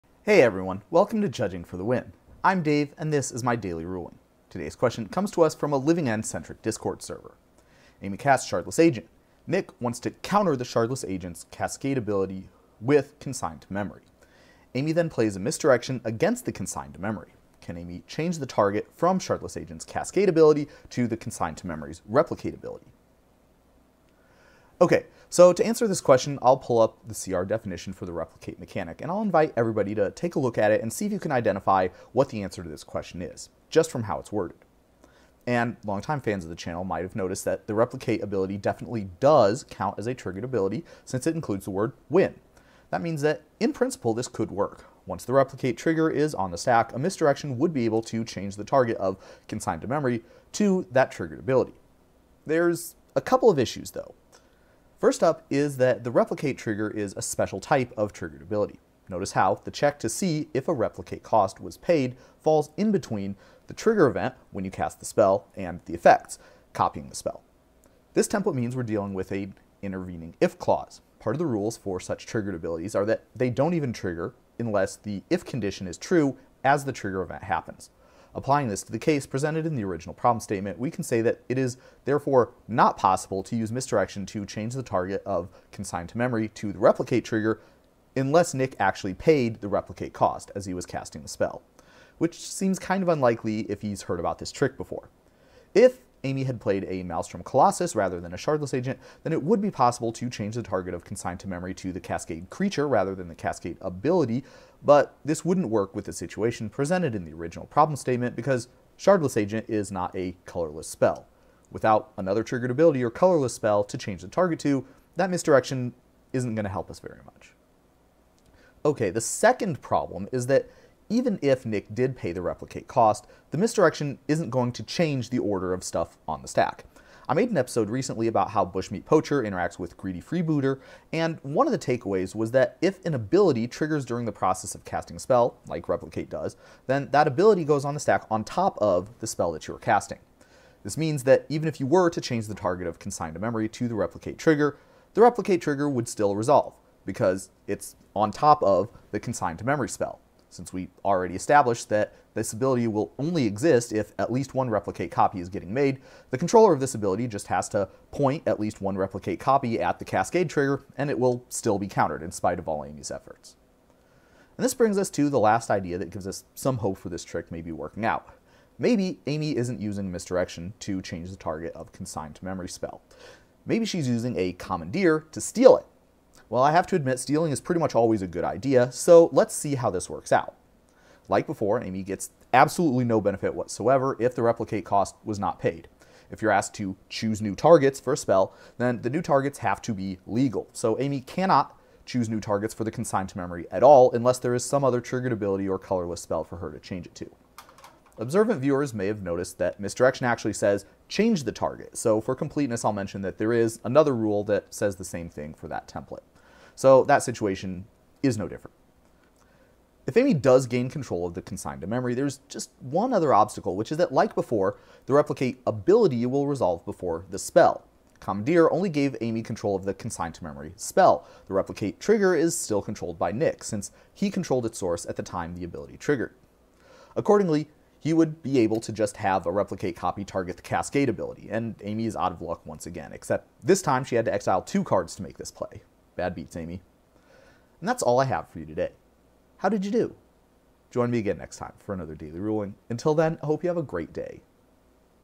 DDR#819 - Misdirection vs. Consign to Memory
(3*) Can you save your cascade trigger with a Misdirection?
A: Only if the stars align very precisely